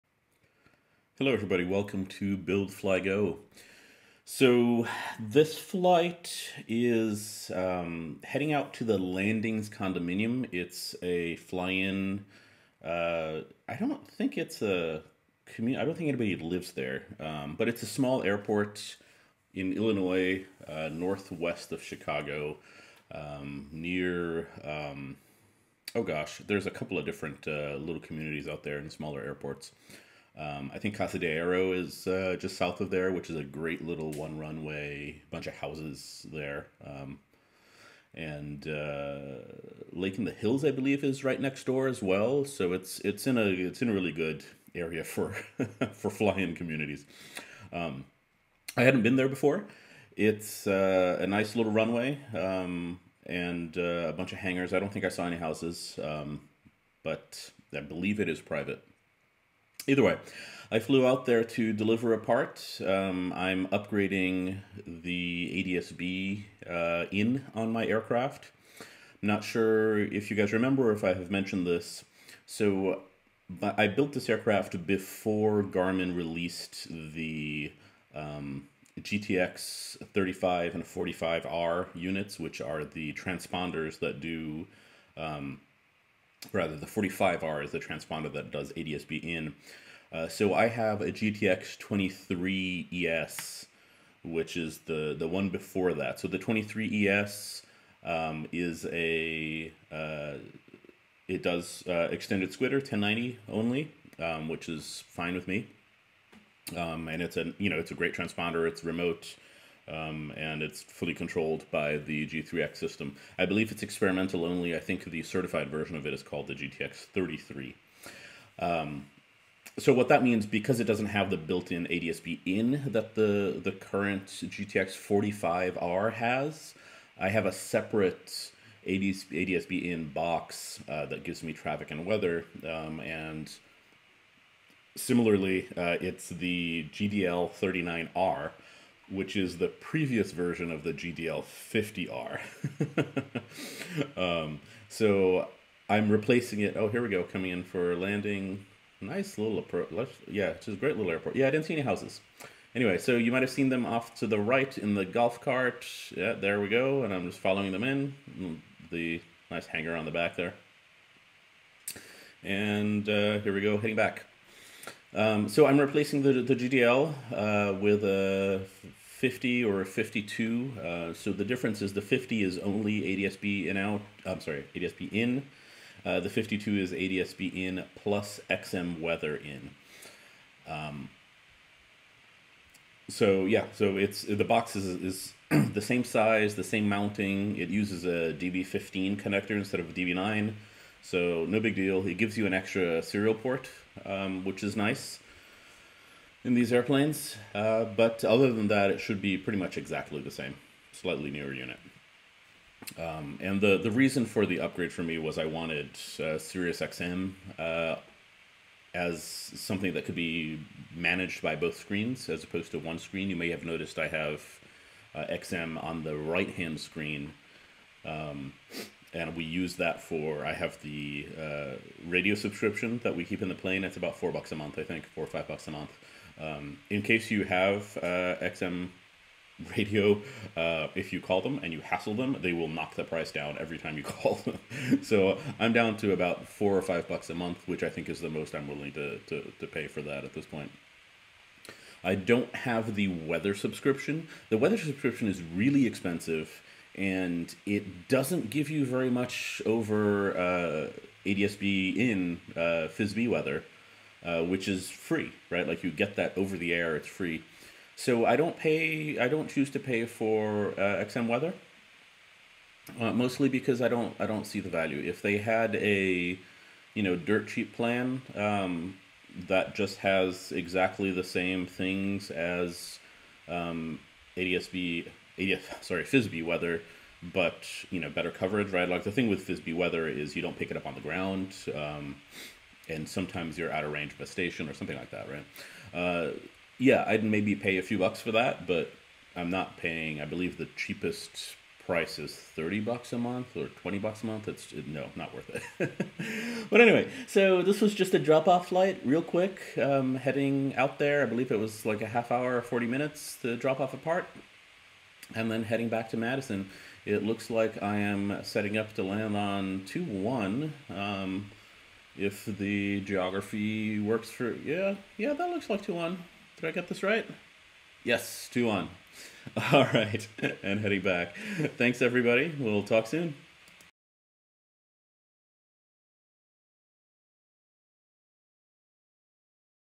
Delivering ADS-B unit & ADS-B vs XM Weather
Flying out to drop off a part I'm replacing and a short discussion on the merits of FIS-B Weather vs XM Weather.